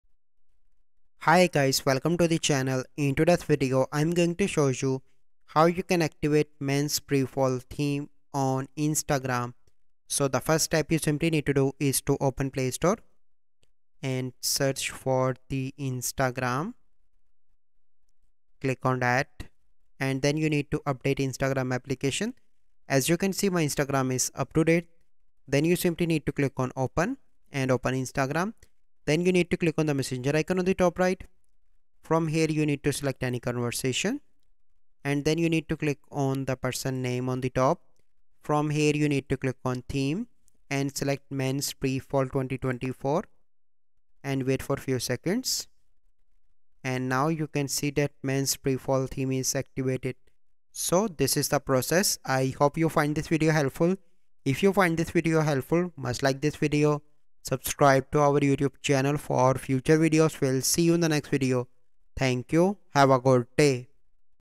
How To Activate Men's Pre Fall 2024 Theme On Instagram (New)
🍂 Elevate Your Instagram Aesthetic: Activating Men's Pre-Fall 2024 Theme! 📸✨

🕶️ Dive into the world of style with our comprehensive guide on activating the Men's Pre-Fall 2024 theme on Instagram! 🍁📲